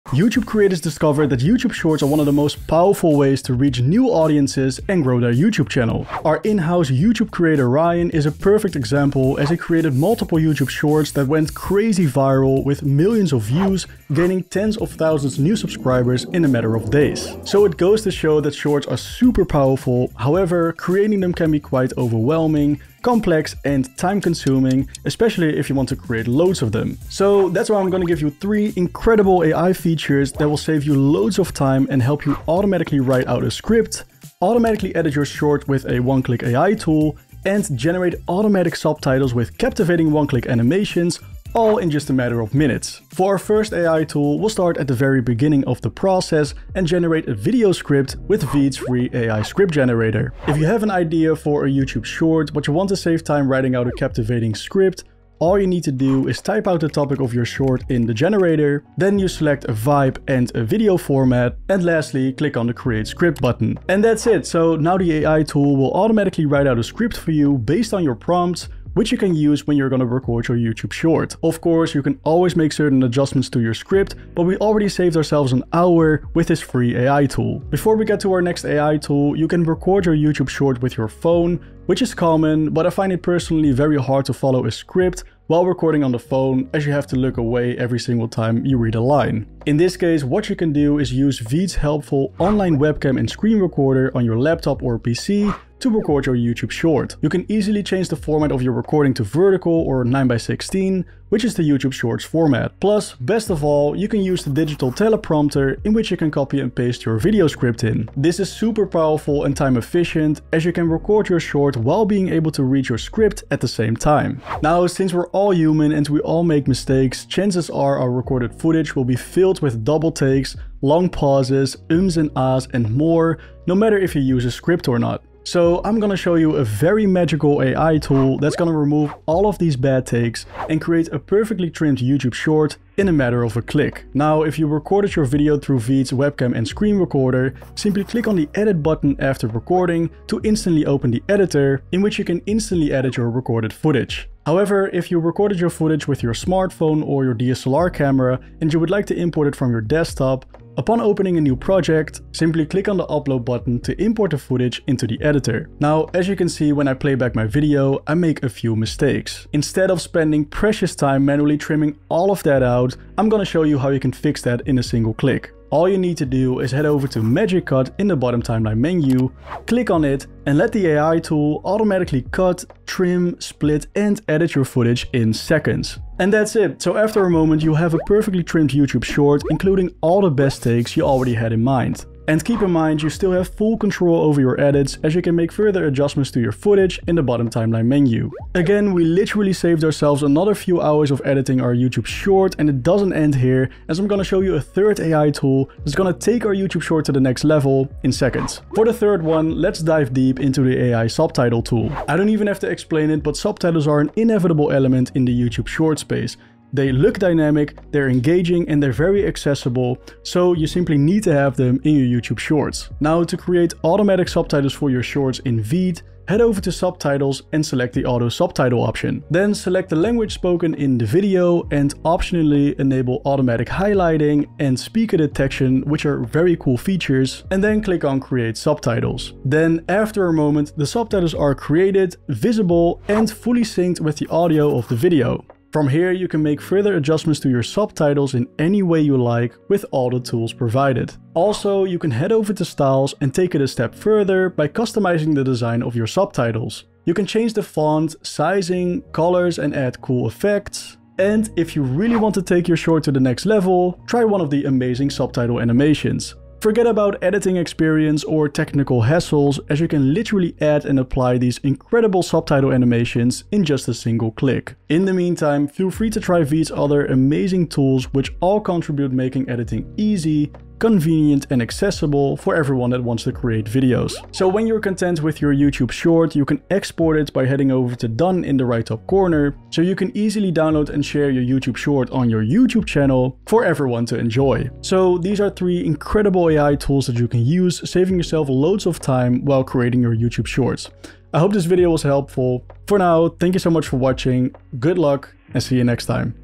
3 AI Tools to Make YouTube Shorts in MINUTES 🚀
In this video, you'll learn how to make YouTube Shorts super easy and quick, with the help of 3 incredible AI video production tools! 🚀

AI helps video creators significantly reduce time on creating the videos they want. It's not any different when creating YouTube Shorts... from literally writing out a script, to editing, to creating subtitles. ⏱️

Doing all of this manually would take HOURS! That's why we give you 3 helpful AI tools to make YouTube Shorts from beginning to end, all powered by VEED's simple online video editor. 🙌

The first AI tool is VEED's FREE AI Video Script Generator. This tool allows you to turn an idea into a full-blown video script for a YouTube Short in a matter of moments. 📝

TIP: Use VEED's online webcam and screen recorder to record a YouTube Short with, as you can use the built-in teleprompter you can paste your auto-generated video script in! 🎥

The second AI tool is Magic Cut, VEED's AI-powered editing tool that automatically turns your raw footage into a perfectly trimmed YouTube Short... in a single click! The AI tool removes double takes, filler words and awkward silences, while creating a cut YouTube Short with all the best takes included. 🪄

The third AI tool is VEED's AI Subtitle tool, which auto-captions your YouTube Short in seconds. You can fully customize the design of the subtitles for your Short, and even add captivating one-click subtitle animations to really make the video stand out! 🚀

After saving all this time and getting the tedious tasks out of the way with the help of these AI tools,  you can focus on the fun and creative stuff! In VEED's editor, you have endless possibilities to further spice up your Shorts. Think about animated text, audio visualizers, stickers, icons, GIFs, audio and video stock footage, and much more. 🔥

Try these magical AI tools for yourself and save yourself hours from making YouTube Shorts by clicking on one of the links above! 🎉

🚀  How to Make YouTube Shorts in minutes (using 3 AI tools):

00:00 Intro
00:27 Video overview
00:43 #1: AI Video Script Generator for YouTube Shorts
01:18 TIP: How you can record your YouTube Short
01:54 #2: Magic Cut (AI) - ONE-click edit your YouTube Short
03:14 #3 AI Subtitle - Auto-caption your YouTube Shorts
04:25 Optional: Use VEED's other amazing tools for your YT Short!
04:34 How to export YouTube Short
04:45 Try these 3 AI tools and SAVE TIME making YT Shorts! 🚀

Tim & VEED.IO